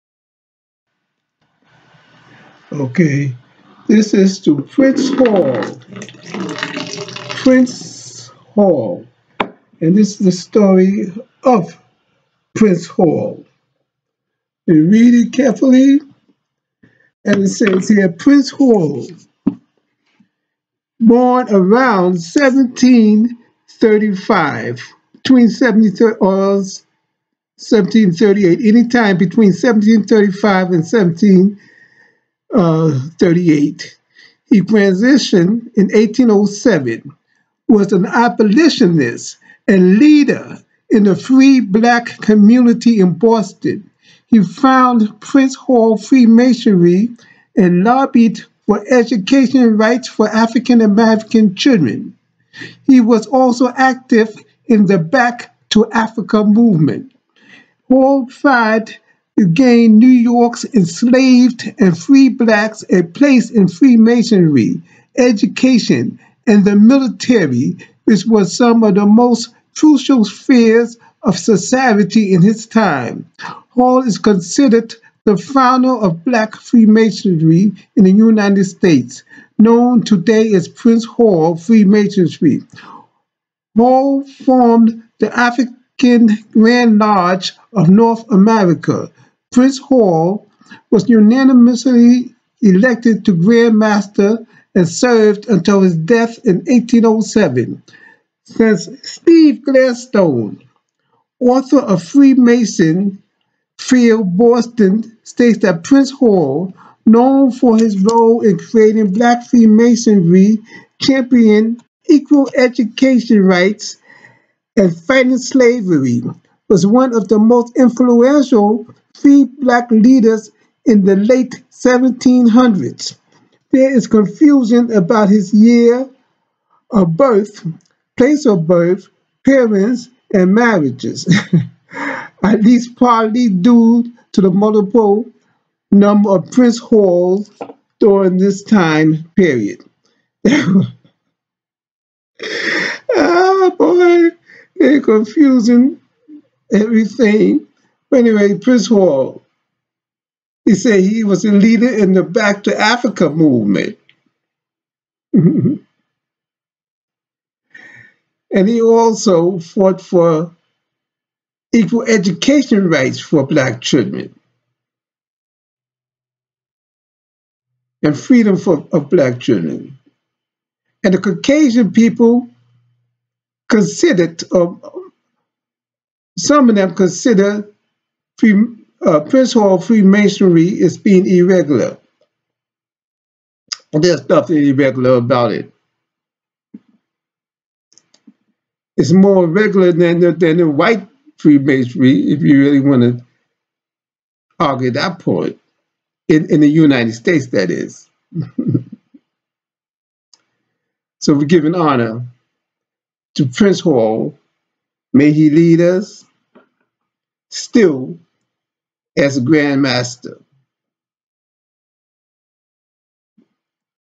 PRINCE HALL
Prince Hall (c.1735/8 - 1807) was  an abolitionist and leader in the free Black community in Boston. He founded Prince Hall Freemasonry and lobbied for education rights for African-American children. He was also active in the Back-to- Africa movement.